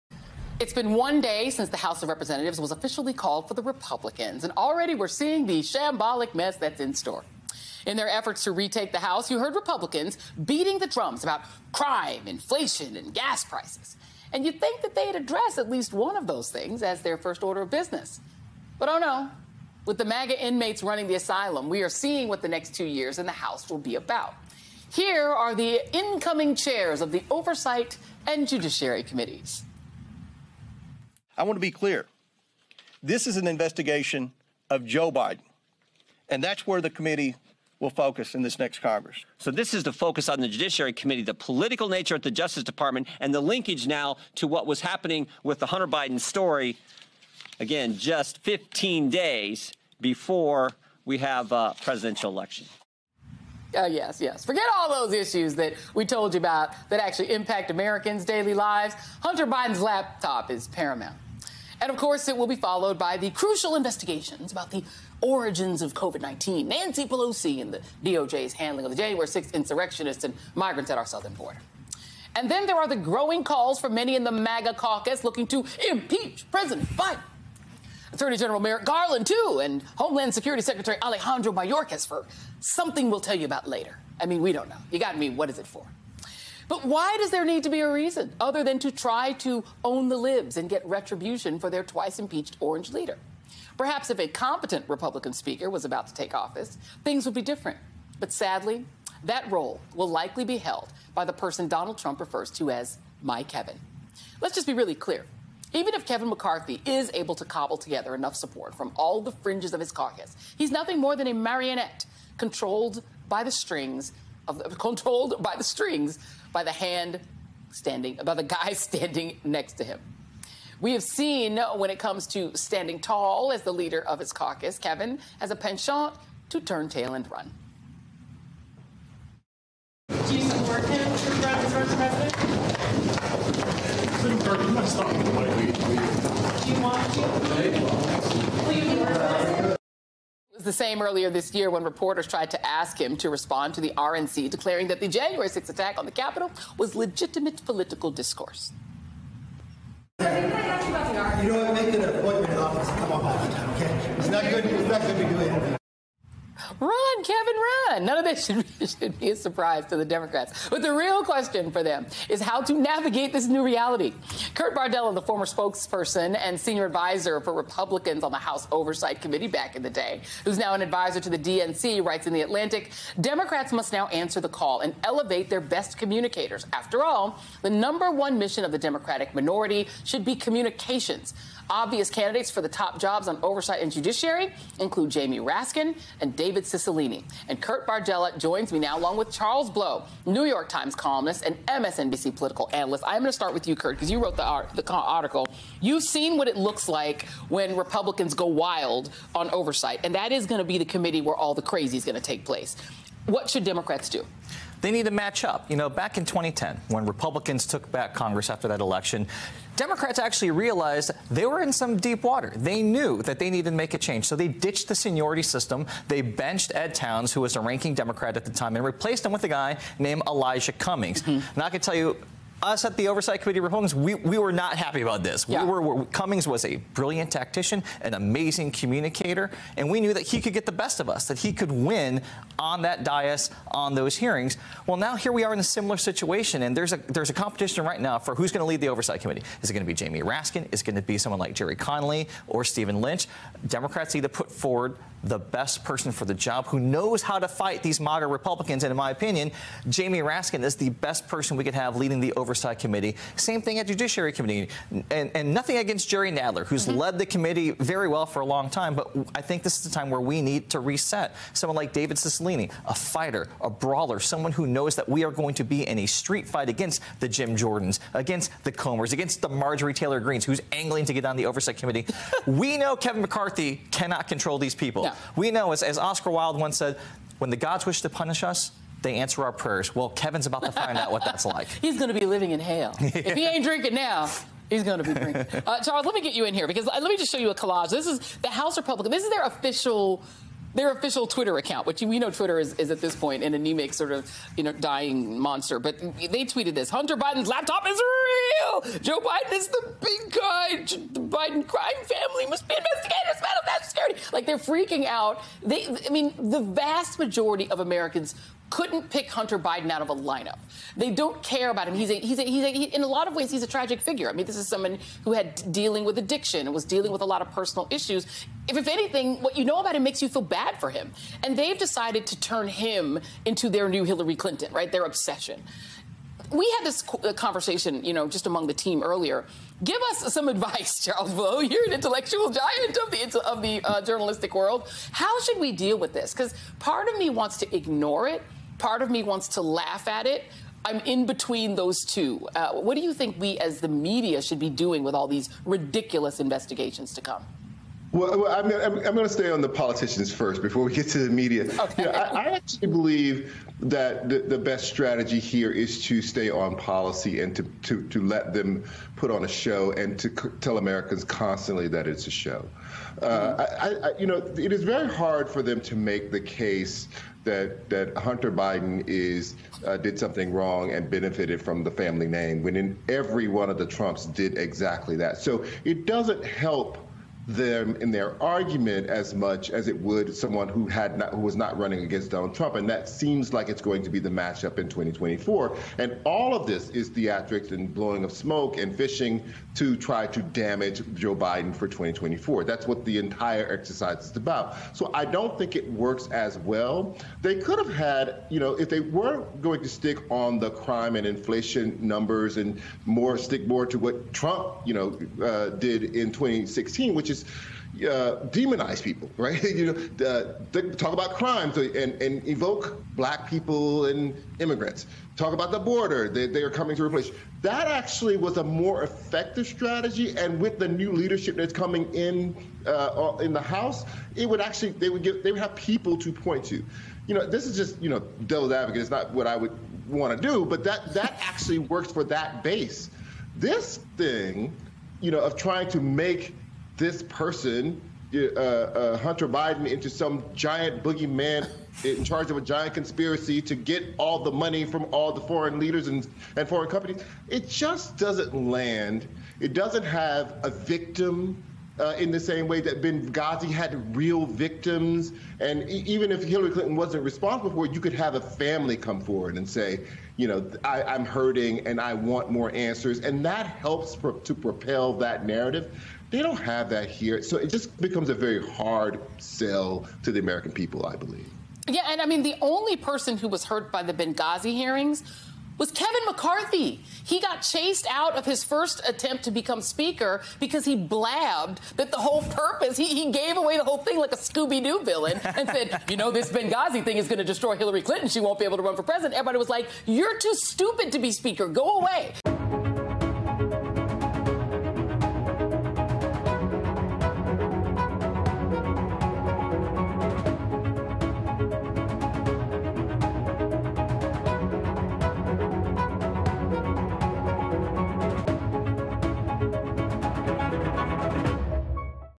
Kurt Bardella On House GOP: We Know Kevin McCarthy Cannot Control These People
House Republicans are apparently more focused on Joe and Hunter Biden in the lead up to their assuming the majority in Congress, instead of inflation and crime, two issues that they made prominent during the midterm elections. Joy Reid and her panel discuss how Kevin McCarthy will likely not be able to control the investigative onslaught many prominent Republicans reportedly have planned.

Republicans have won control of the House by a slim margin. With Democrats gaining control of the Senate, President Biden will now need to work with a divided congress.